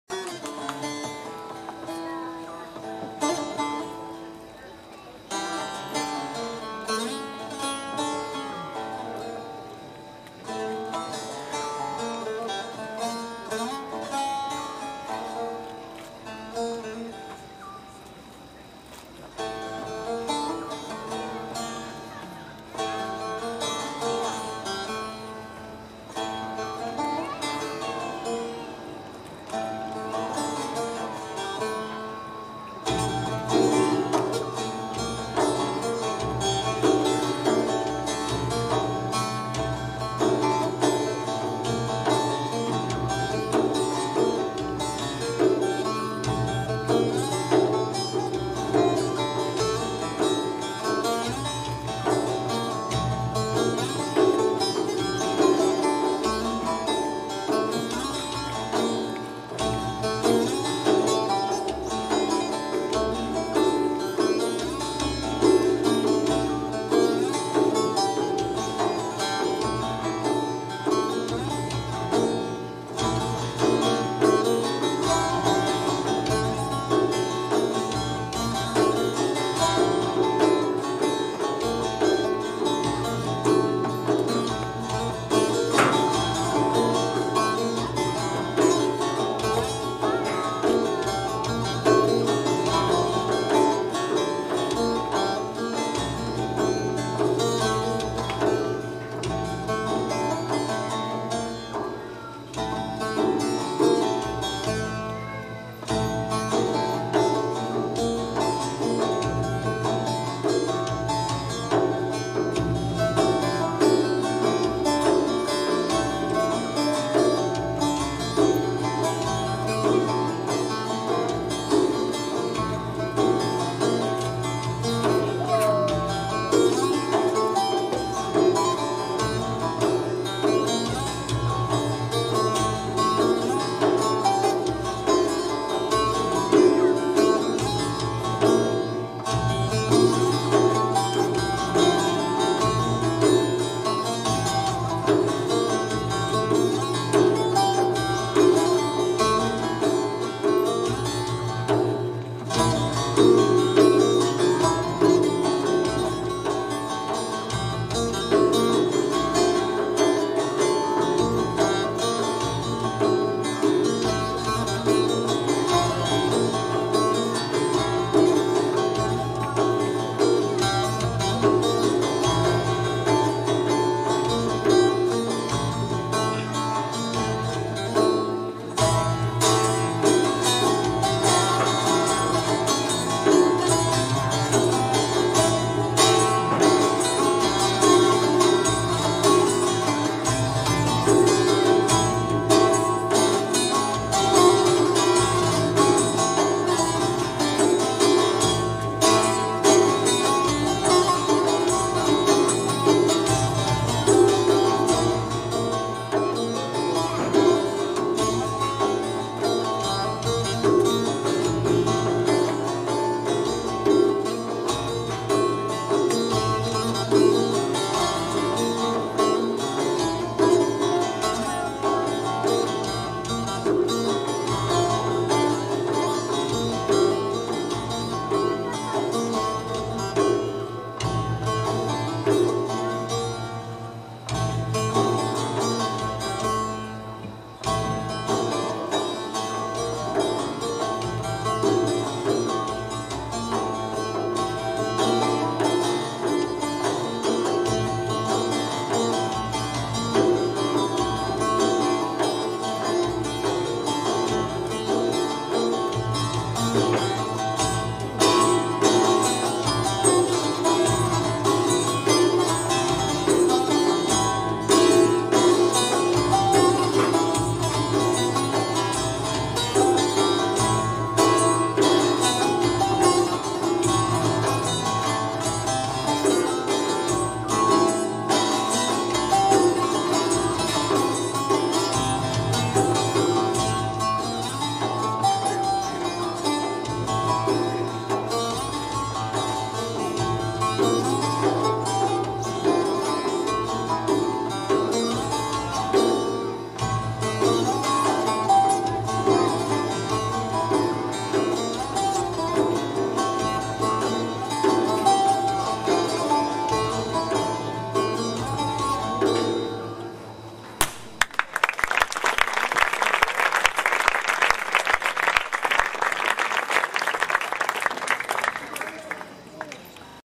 OUR FESTIVAL 29 Ιουλίου WEAVING WORLDS Φεστιβάλ Ρεματιάς 2020 Νύχτες Αλληλεγγύης
Ο Paul Goodman στο σάζι και τη λύρα, με τη συνοδεία του Αλέξη Νόνη στα κρουστά, παρουσίασε προσωπικές του συνθέσεις που συνδυάζουν τους ήχους των παλιών τρόπων και οργάνων, εκφέροντας ένα σύγχρονο άκουσμα οργανικής μουσικής.